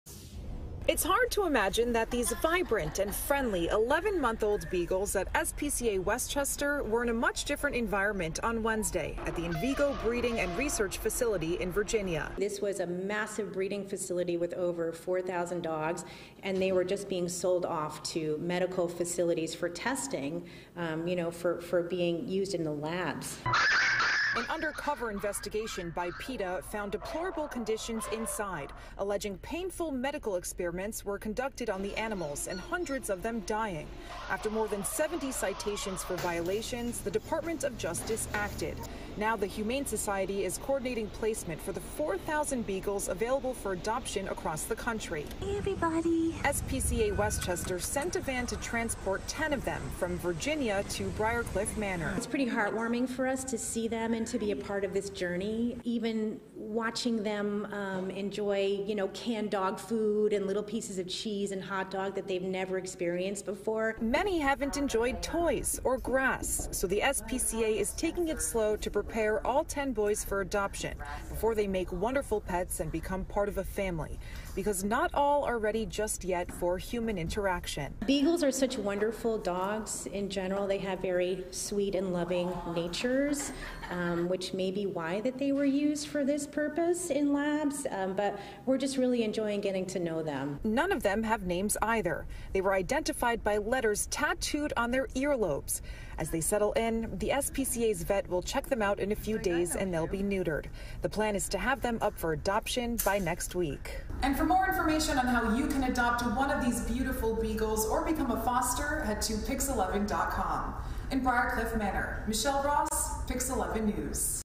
10 beagles rescued from VA research facility now in NY
The dogs are part of a group of 4,000 beagles saved from being sold for medical testing in laboratories.